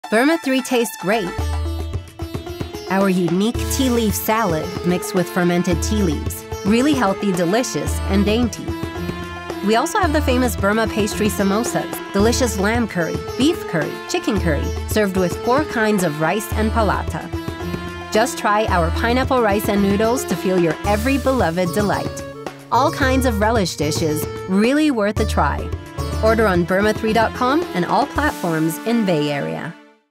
Burma 3 - Authentic burmese takeout in the south bay | tea leaf salad | samosa | pineapple rice
Burma 3 tastes great!
A variety of grains and vegetables mixed with fermented tea leaves.
Our unique tea leaf salad is really healthy, delicious, and dainty.
We also have the famous Burma pastry samosas, delicious lamb curry, beef curry and chicken curry.
Served with four kinds of rice and palata.
Just try our pineapple rice and noodles to feel your every beloved delight.
All kinds of relish dishes are really worth a try.